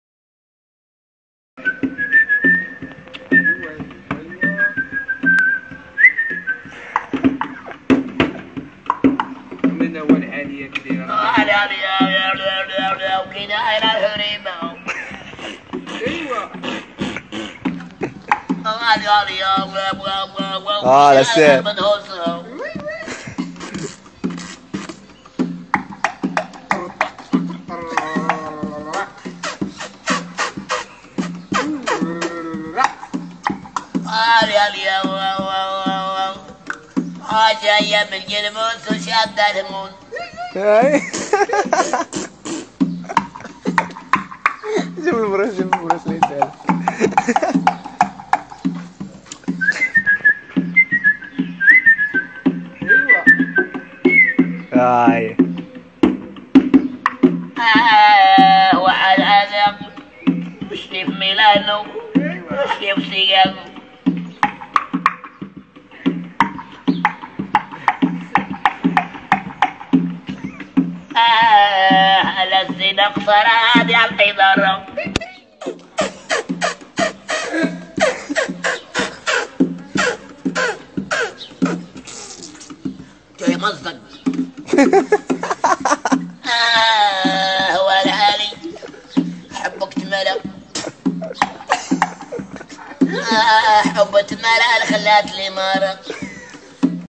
الموهبة المتشردة
صوت بگام  حسن و موهبة في الرنة مع مع اداء موسيقى اختيارية = موهبة ضائعة متشردة